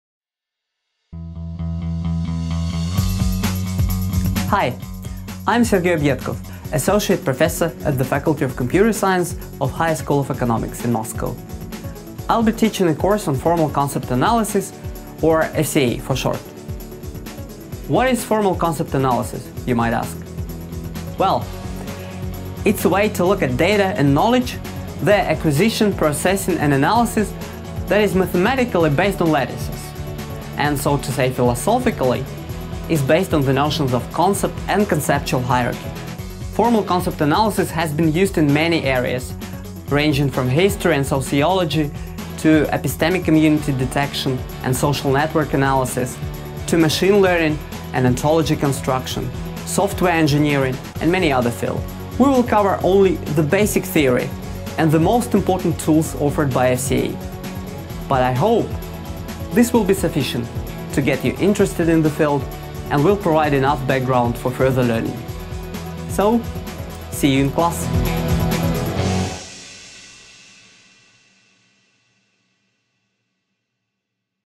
Introduction to Formal Concept Analysis
Institution: National Research University Higher School of Economics

Course: Introduction to Formal Concept Analysis

About the Course: This course is an introduction into formal concept analysis (FCA), a mathematical theory oriented at applications in knowledge representation, knowledge acquisition, data analysis and visualization. It provides tools for understanding the data by representing it as a hierarchy of concepts or, more exactly, a concept lattice. FCA can help in processing a wide class of data types providing a framework in which various data analysis and knowledge acquisition techniques can be formulated. In this course, we focus on some of these techniques, as well as cover the theoretical foundations and algorithmic issues of FCA. Upon completion of the course, the students will be able to use the mathematical techniques and computational tools of formal concept analysis in their own research projects involving data processing. Among other things, the students will learn about FCA-based approaches to clustering and dependency mining.

Taught by: Sergei Obiedkov , Associate Professor, HSE